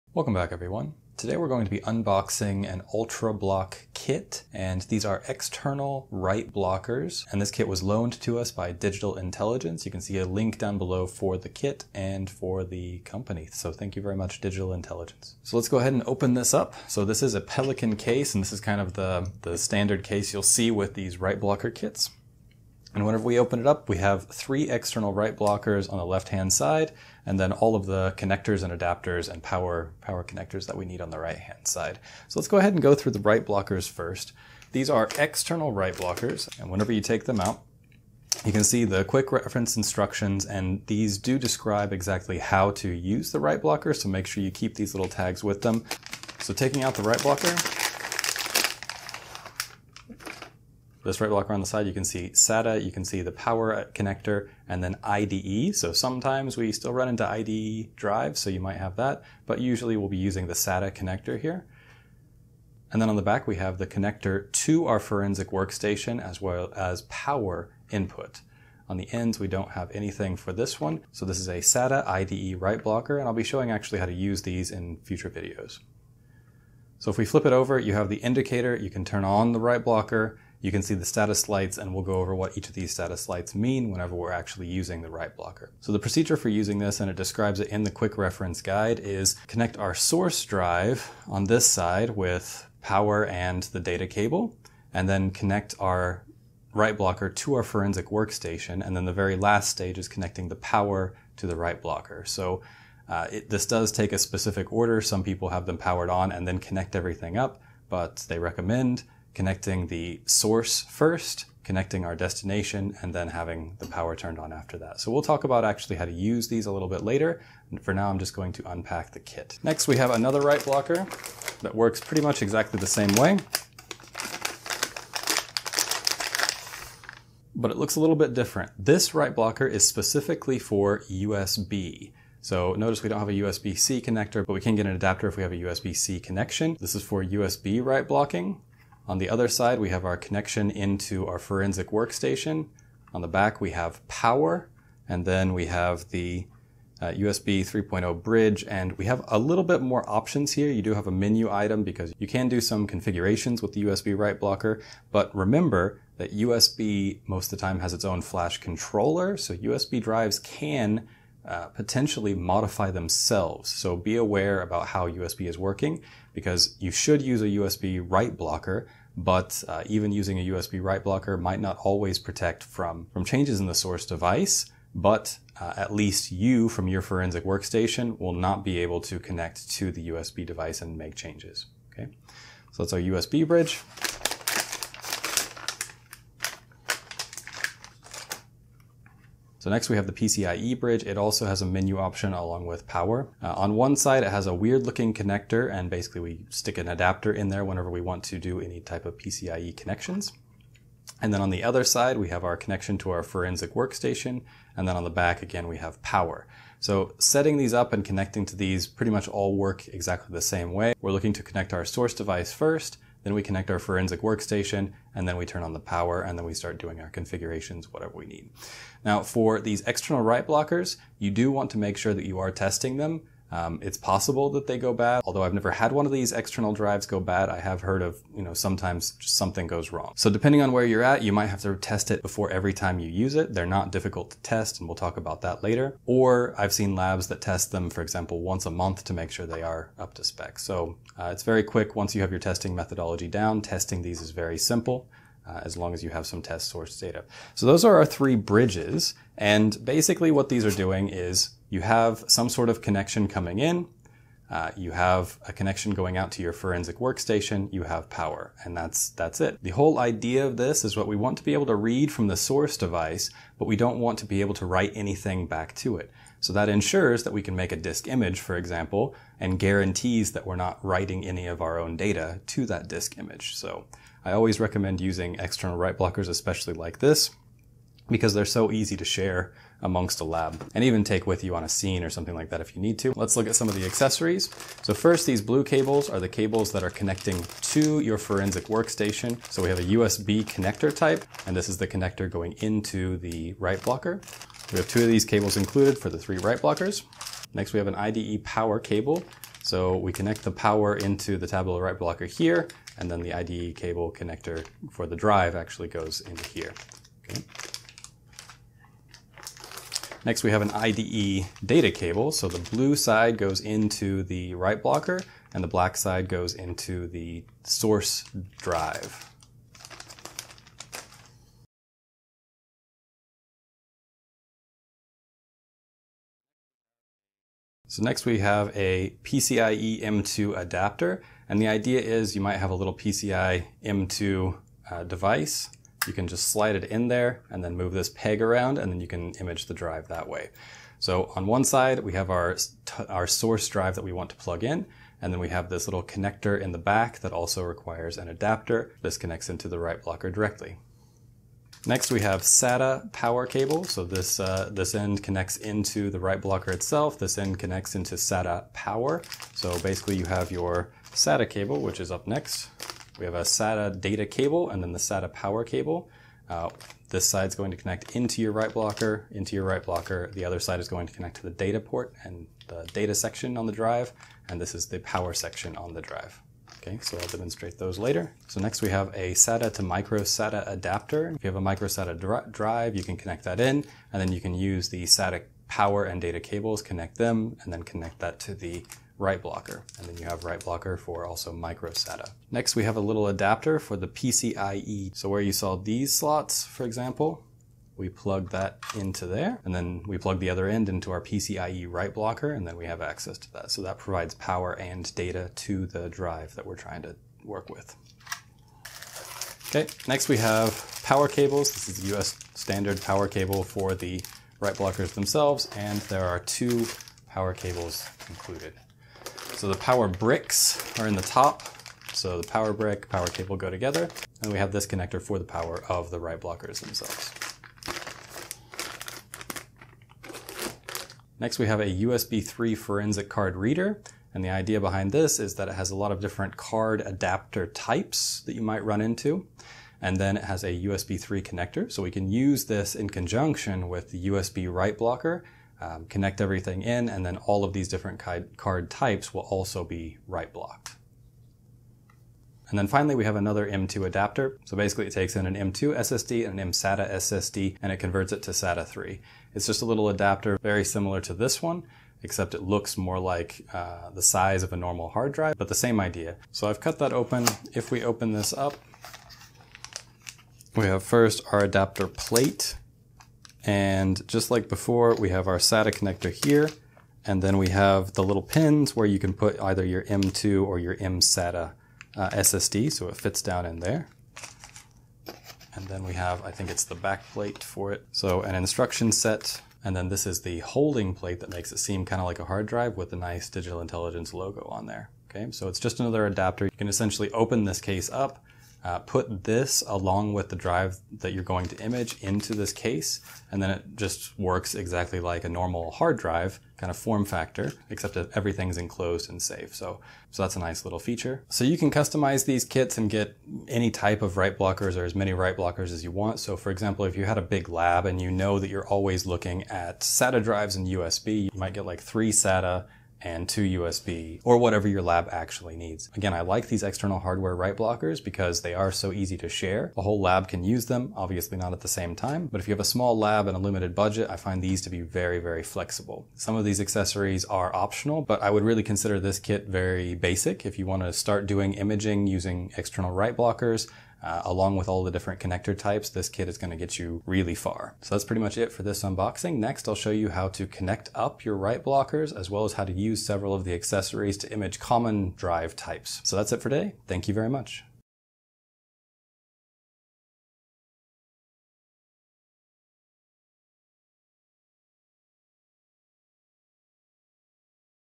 What's in the box? Digital Intelligence UltraBlock Kit!
Unboxing an UltraBlock external forensic write block kit from Digital Intelligence. The UltraBlock write blockers and Tableau TX1 Imager was generously loaned by Digital Intelligence, Inc.

Thank you to our Members and Patrons, but especially to our Investigators, TheRantingGeek, Roman, and Alexis Brignoni! Thank you so much!

00:00 UktraBlock External Write Blocker Kit
00:15 UltraBlock kit unboxing
00:45 UltraBlock SATA/IDE
01:25 Hardware write blocker standard procedure
01:59 UltraBlock USB external write blocker
03:03 UltraBlock PCIe external write blocker
03:38 A note on write blocker testing
04:43 UltraKit Accessories
08:31 UltraBlock Kit Conclusions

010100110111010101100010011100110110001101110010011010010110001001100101

Please link back to the original video. We would love to see what you are doing.